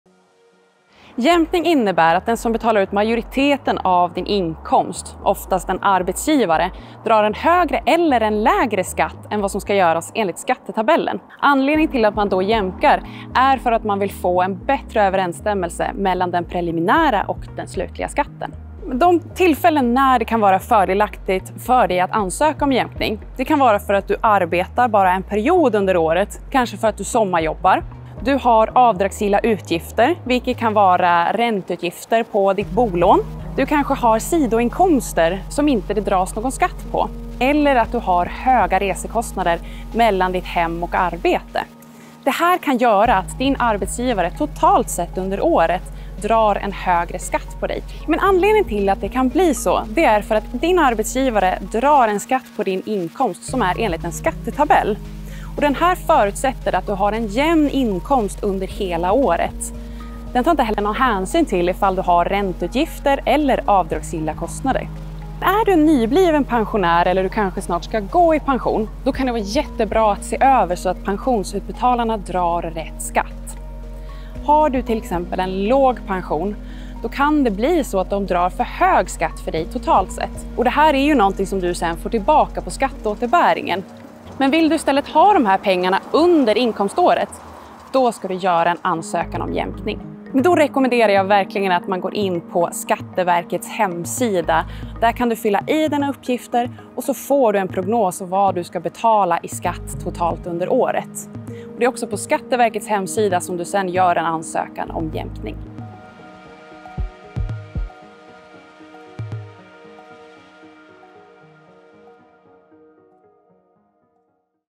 Då ska du ansöka om jämkning på skatten
Din arbetsgivare betalar in din inkomstskatt till staten enligt en skattetabell, men ibland ser verkligheten lite annorlunda ut. För att skatteinbetalningarna ska bli så korrekta som möjligt kan du ansöka om jämkning. Elin Fornbrandt, rådgivare på Stadshypotek, förklarar hur det fungerar.

EFN Ekonomikanalen finns nu även som app!